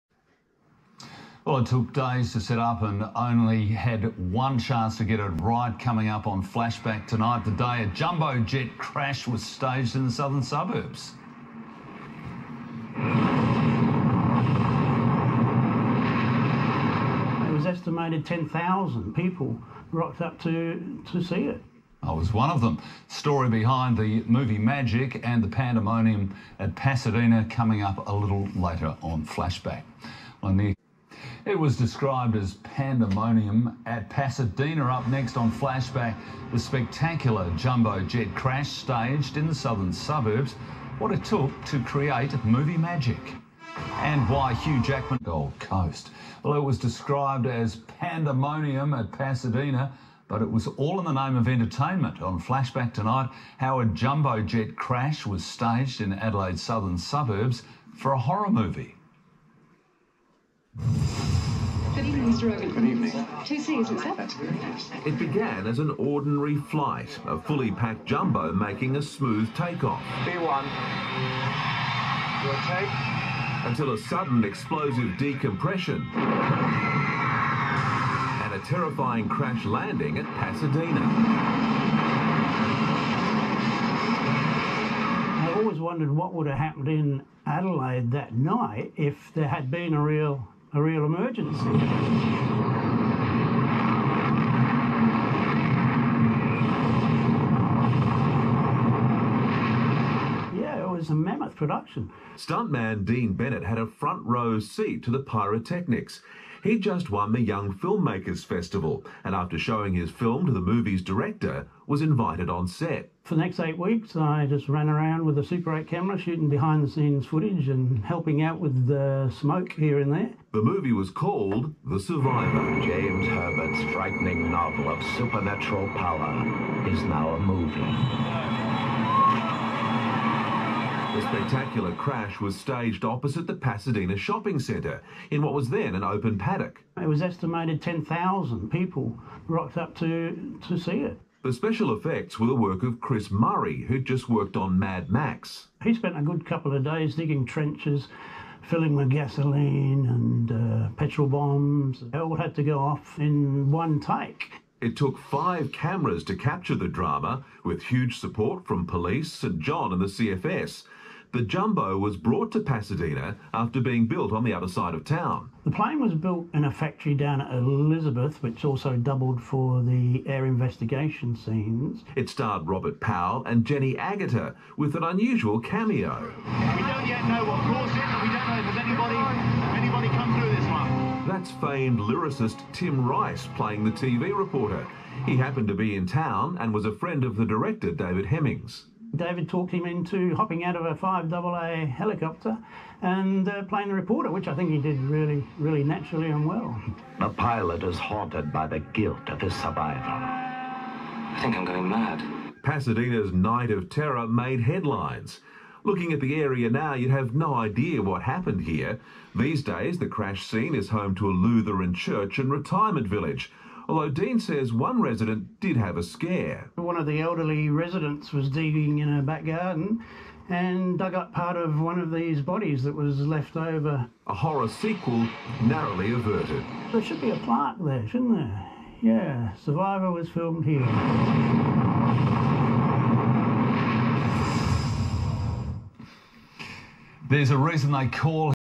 THE SURVIVOR. 1980. Flashback News Story. Interview with Dean Bennett.
Channel 7 story on the movie, The Survivor. Filmed in Adelaide, South Australia. Interview with ME.